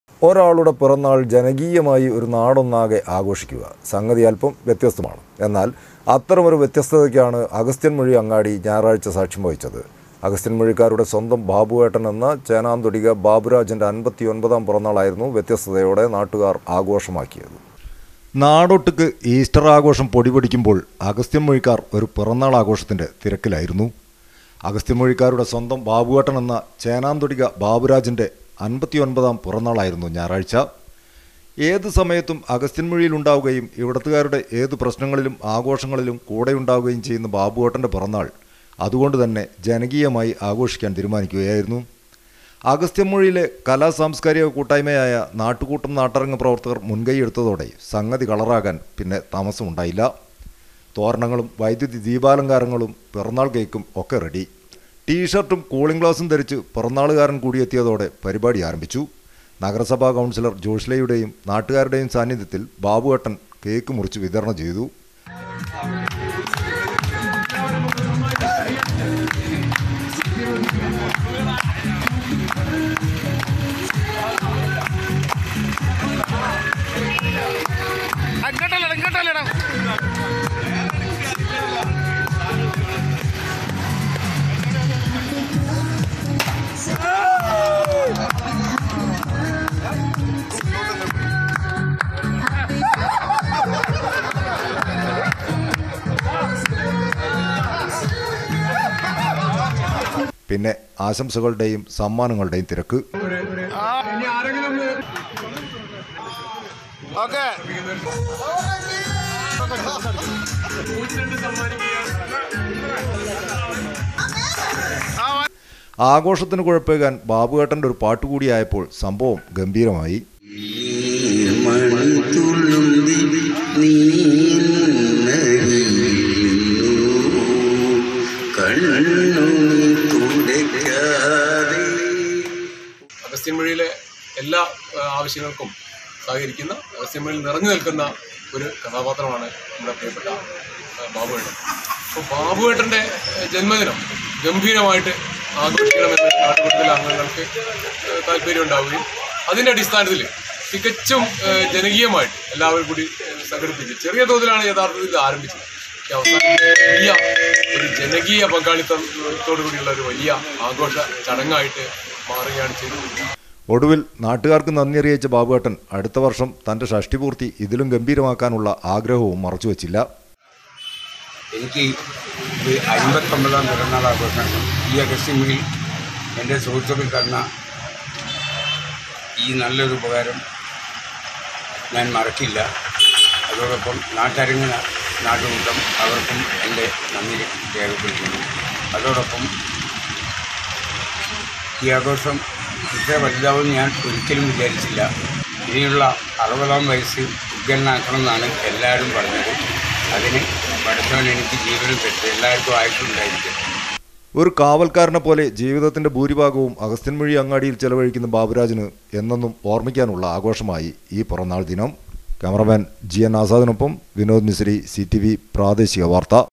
ഒരാളുടെ പിറന്നാൾ ജനകീയമായി ഒരു നാടൊന്നാകെ ആഘോഷിക്കുക
ഒരാളുടെ പിറന്നാൾ ജനകീയമായി ഒരു നാടൊന്നാകെ ആഘോഷിക്കുക. സംഗതി അൽപ്പം വ്യത്യസ്തമാണ്. എന്നാൽ അത്തരമൊരു വ്യത്യസ്തതക്കാണ് അഗസ്ത്യന്മുഴി അങ്ങാടി ഞായറാഴ്ച സാക്ഷ്യം വഹിച്ചത്. അഗസ്ത്യന്മുഴിക്കരുടെ സ്വന്തം ബാബുവേട്ടൻ എന്ന ചേനാംതൊടിക ബാബുരാജന്റെ അൻപത്തിഒൻപതാം പിറന്നാളായിരുന്നു വ്യത്യസ്തതയോടെ നാട്ടുകാർ ആഘോഷമാക്കിയത്